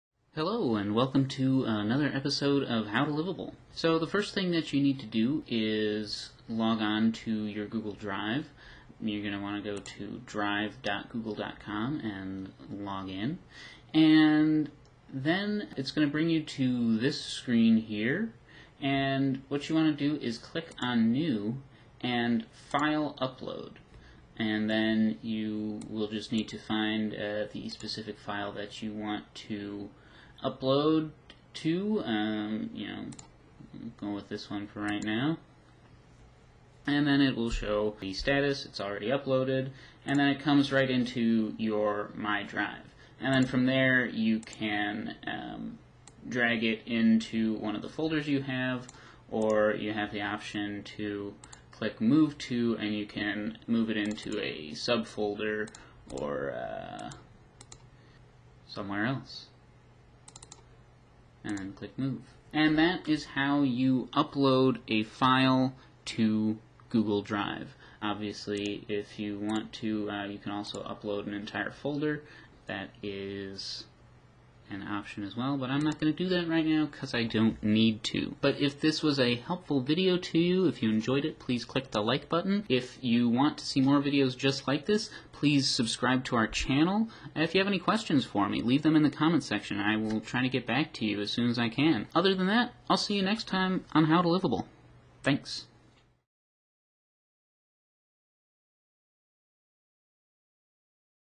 How to Upload Files to Google Drive
To upload files to Google drive:
- Log in on to Google drive and click on New, File Upload
- Find the file that you want to upload, click Open
- You will see the status of your upload on your drive
- You can drag and drop a file on any of your folder
- You can also right click, click Move to and choose the folder you where you want to move your file